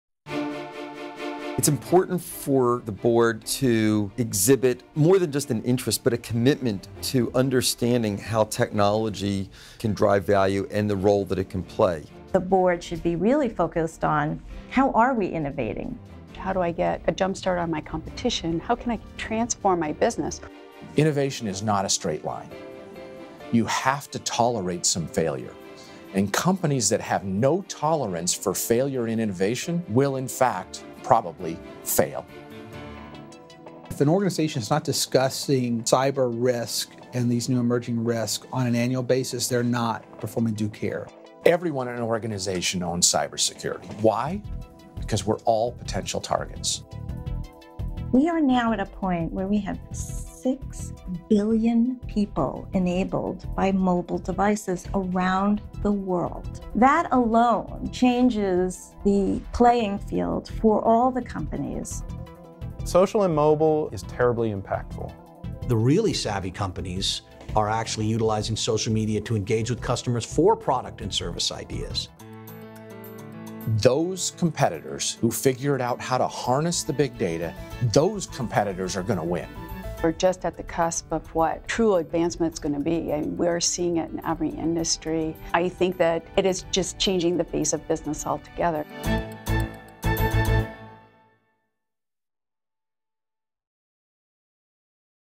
The Intersection of Technology, Strategy, and Risk: a video series defining the board's role.
As information technology (IT) continues to evolve, so does the role of an effective corporate director. From big data analytics to social media to cybersecurity, technology creates opportunities for companies to innovate, to create operational efficiencies, and to develop a competitive advantage.